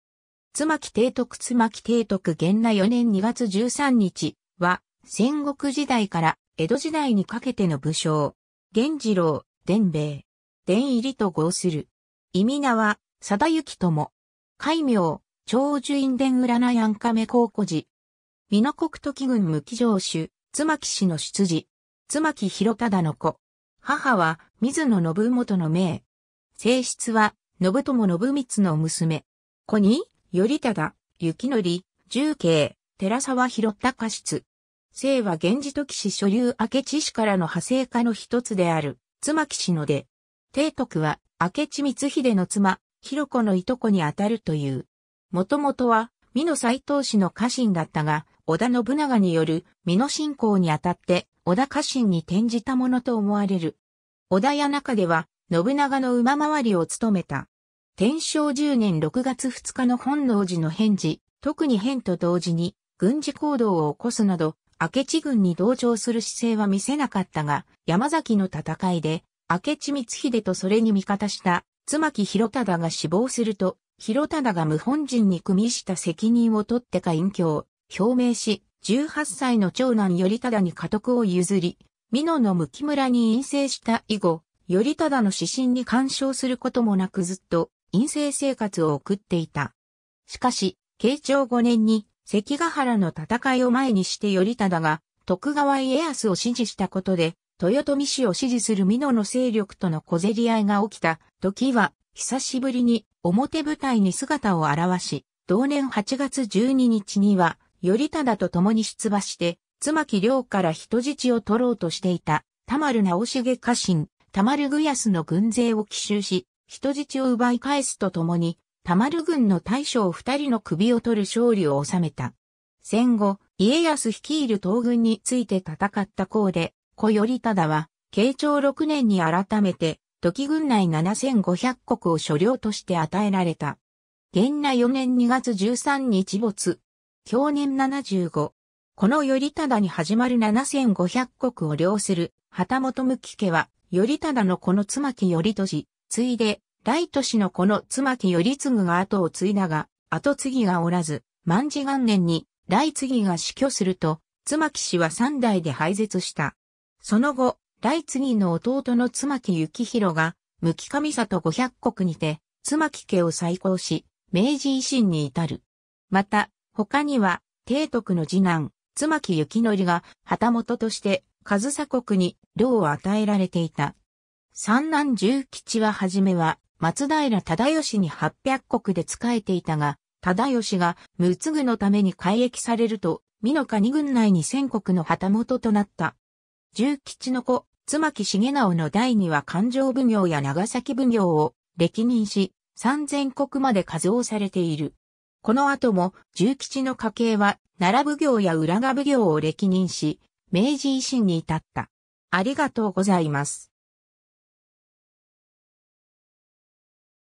妻木貞徳
妻木貞徳 妻木 貞徳（つまき さだのり、天文13年（1544年） - 元和4年2月13日（1618年3月9日））は、戦国時代から江戸時代にかけての武将。
源二郎、伝兵衛。
伝入と号する。
諱は「貞行」とも。
戒名・長寿院殿卜庵亀公居士。
 美濃国土岐郡妻木城主・妻木氏の出自。
妻木広忠の子。
母は水野信元の姪。
正室は延友信光（土岐三兵某）の娘。
子に頼忠、之徳、重慶、寺沢広高室。
 清和源氏土岐氏庶流明智氏からの派生家の一つである妻木氏の出。
貞徳は明智光秀の妻、煕子の従兄弟にあたるという。
 元々は美濃斎藤氏の家臣だったが織田信長による美濃侵攻にあたって織田家臣に転じたものと思われる。
織田家中では信長の馬廻を務めた。
 天正10年（1582年）6月2日の本能寺の変時、特に変と同時に軍事行動を起こすなど明智軍に同調する姿勢は見せなかったが、山崎の戦いで明智光秀とそれに味方した妻木広忠が死亡すると、広忠が謀反人に与した責任を取ってか隠居を表明し、18歳の長男頼忠に家督を譲り美濃の妻木村に隠棲した 以後、頼忠の指針に干渉する事もなくずっと隠棲生活を送っていた。
しかし、慶長5年（1600年）に関ヶ原の戦いを前にして頼忠が徳川家康を支持した事で、豊臣氏を支持する美濃の勢力との小競り合いが起きたときは久しぶりに表舞台に姿を表し、同年8月12日には頼忠と共に出馬して、妻木領から人質を取ろうとしていた田丸直昌家臣田丸具安の軍勢を奇襲し、人質を奪い返すと共に田丸軍の大将2人の首を取る勝利を収めた。
戦後、家康率いる東軍について戦った功で子頼忠は慶長6年（1601年）に改めて土岐郡内7,500石を所領として与えられた。
 元和4年（1618年）2月13日没。
享年75。
 子の頼忠に始まる7,500石を領する旗本妻木家は頼忠の子の妻木頼利、次いで頼利の子の妻木頼次が跡を継いだが、跡継ぎがおらず万治元年（1658年）に頼次が死去すると、妻木氏は3代で廃絶した。
その後、頼次の弟の妻木幸広が妻木上郷500石にて妻木家を再興し、明治維新に至る。
 また、他には貞徳の二男、妻木之徳（頼久）が旗本として上総国に領を与えられていた。
三男重吉（頼通）は始めは松平忠吉に800石で仕えていたが、忠吉が無嗣の為に改易されると美濃可児郡内に1,000石の旗本となった。
重吉の子妻木重直（頼熊）の代には勘定奉行や長崎奉行を歴任し、3,000石まで加増されている。
この後も重吉の家系は奈良奉行や浦賀奉行を歴任し、明治維新に至った。